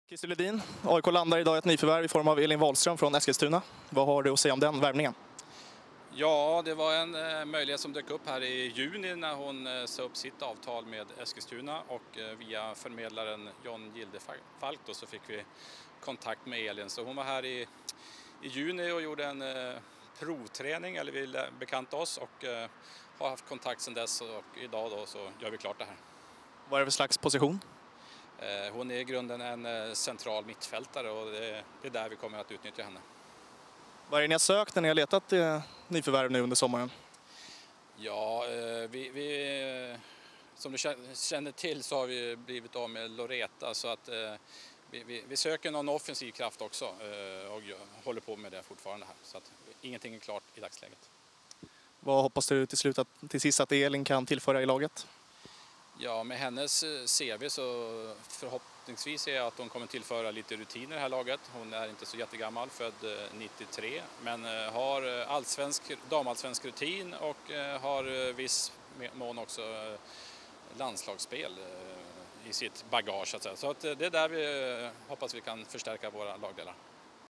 Christer Lundin ger sin syn på värvningen av Elin Wahlström
AIK värvar mittfältaren Elin Wahlström från Eskilstuna United DFF. Hör Sportchef och Chefstränare Christer Lundins ord om nyförvärvet.